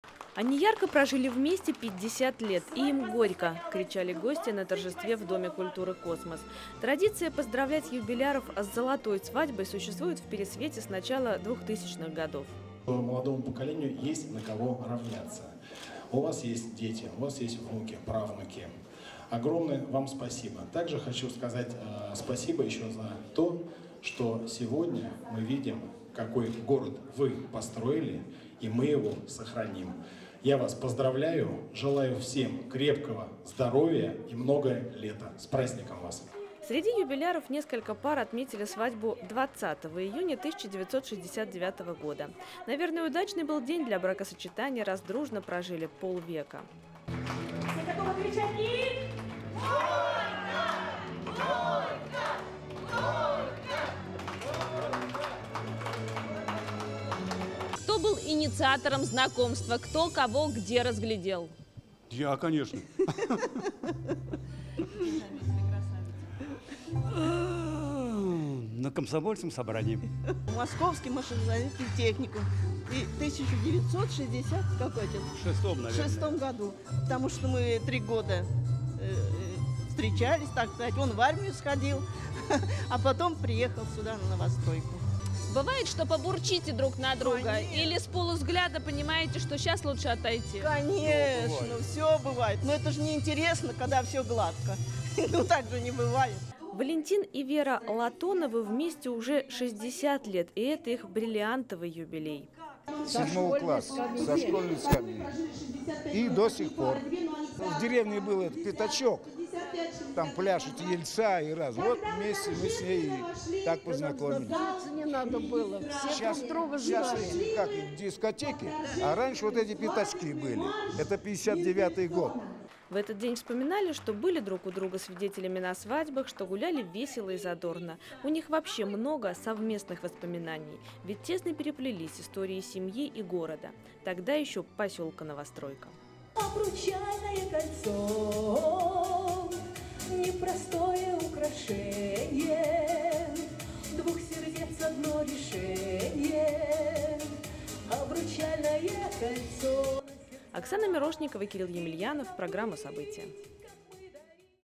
Драгоценные годы | Новости | ТВР24 | Сергиево-Посадский городской округ
Они ярко прожили вместе 50 лет, и им «горько» кричали гости на торжестве в доме культуры «Космос». Традиция поздравлять юбиляров с золотой свадьбой существует в Пересвете с начала 2000-х годов.